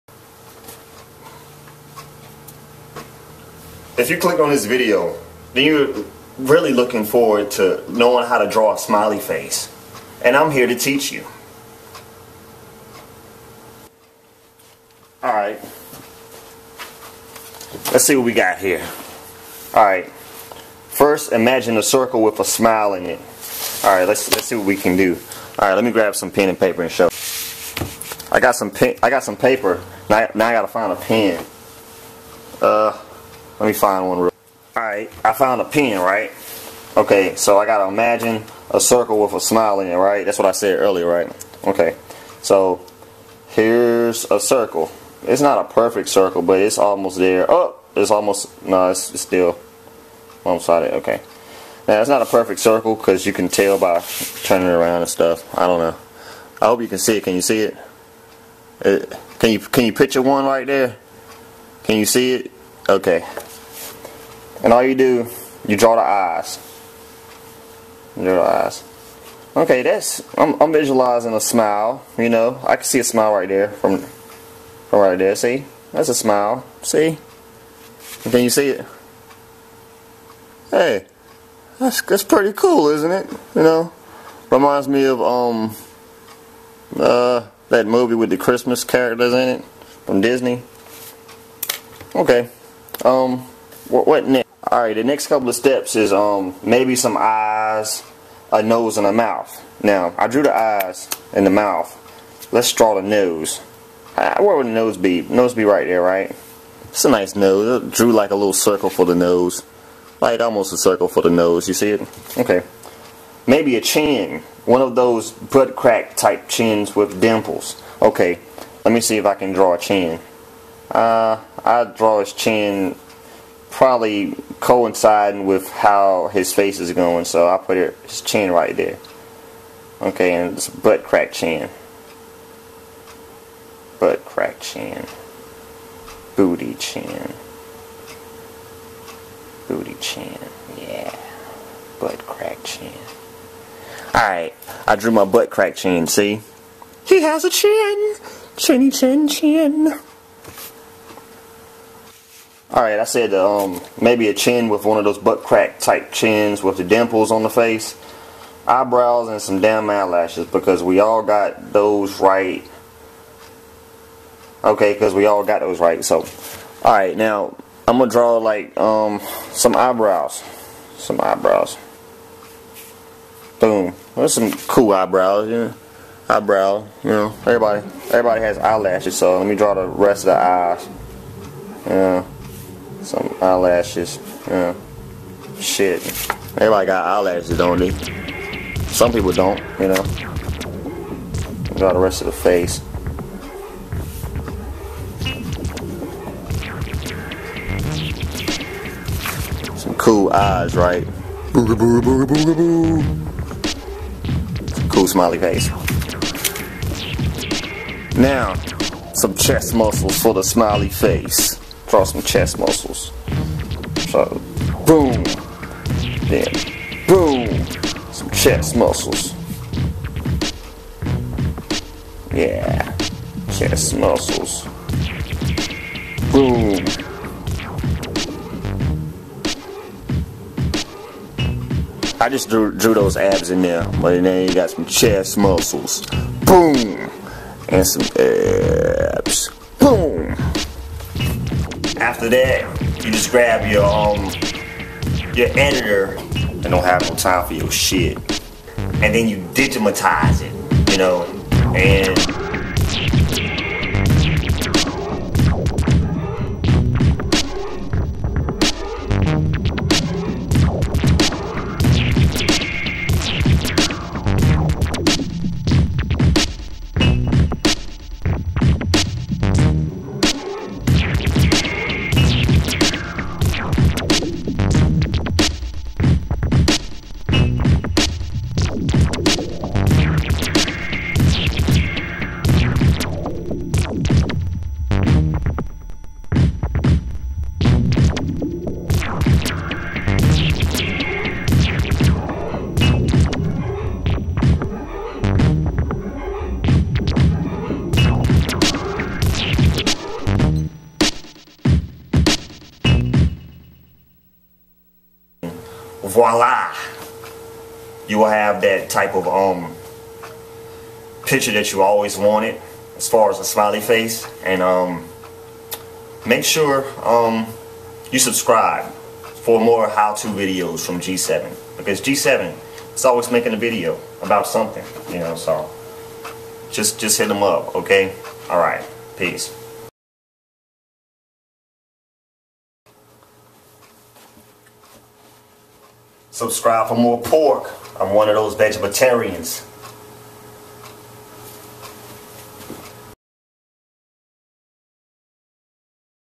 draw a Smiley Face with g7
a simple video of g7thebeatmaker displaying personal creativity ::)